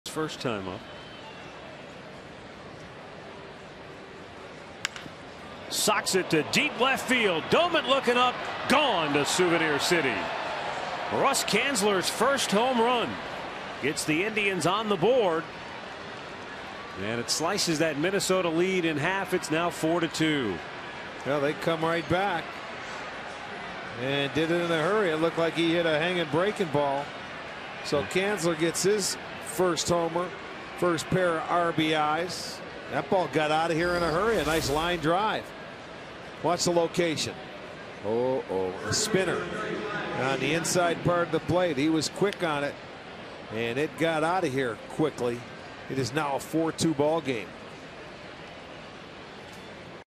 2012/09/07 Canzler's two-run homer
CLE@MIN: Canzler drives a two-run shot to left